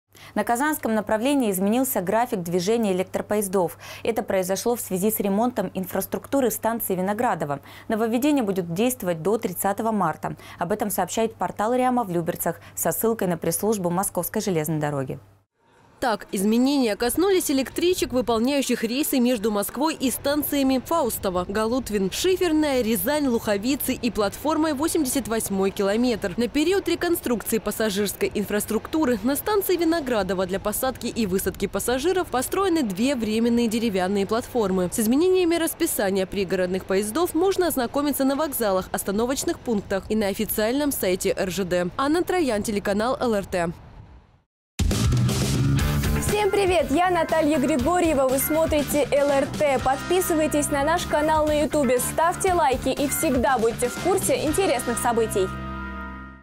На Казанском направлении изменился график движения электропоездов
На Казанском направлении изменился график движения электропоездов. Нововведения будут действовать до 30 марта в связи с ремонтом инфраструктуры станции «Виноградово».

Так, изменения коснулись электричек, выполняющих рейсы между Москвой и станциями «Фаустово», «Голутвин», «Шиферная», «Рязань», «Луховицы» и платформой «88-й километр».

На период реконструкции пассажирской инфраструктуры на станции «Виноградово» для посадки и высадки пассажиров построены 2 временные деревянные платформы.

С изменениями расписания пригородных поездов можно ознакомиться на вокзалах, остановочных пунктах и на официальном сайте «РЖД».

Анна Троян